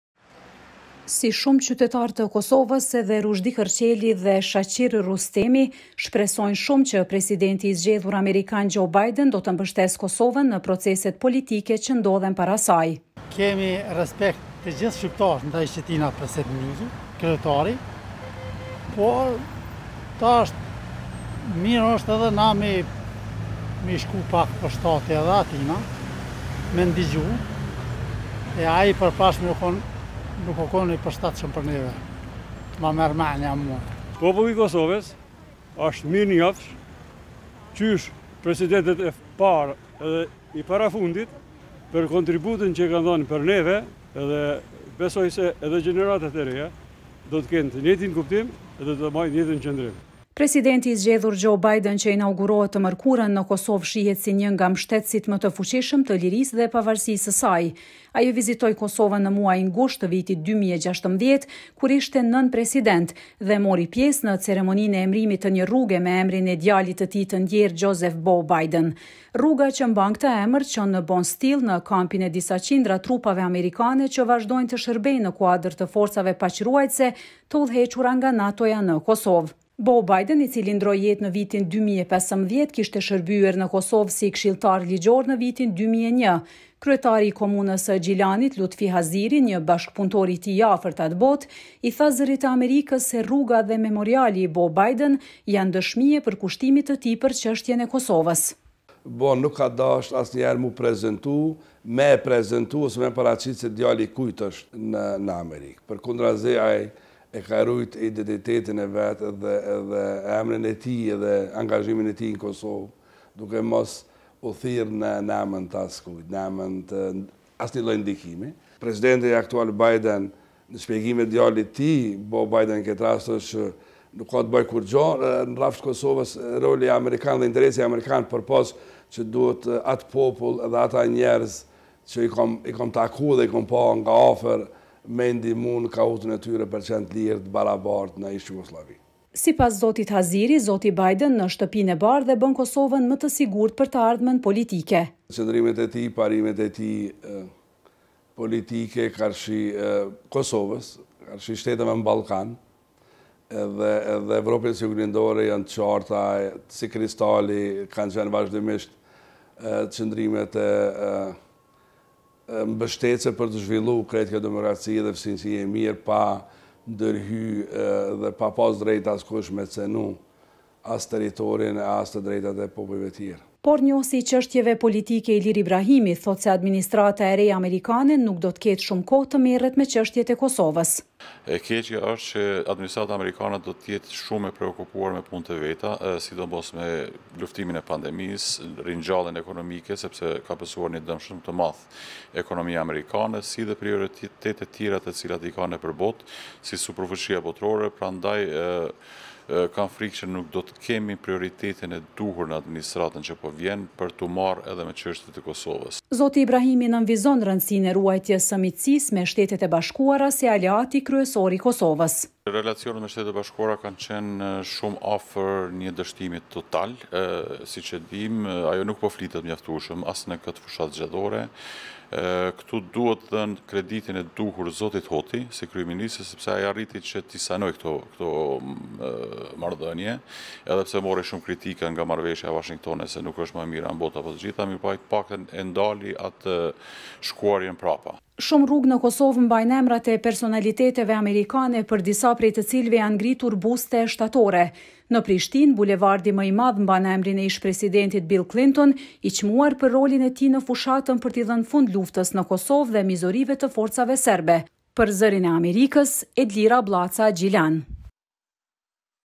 Kosovë: pritshmëritë nga administrata e re amerikane
Në Kosovë po pritet me interes inaugurimi i së mëkures i presidentit të zgjedhur Joe Biden, me shpresën për mbështetje në proceset politike që ndodhen para vendit. Zoti Biden shihet si një nga mbështetësit më të fuqishëm të lirisë dhe pavarësisë së Kosovës.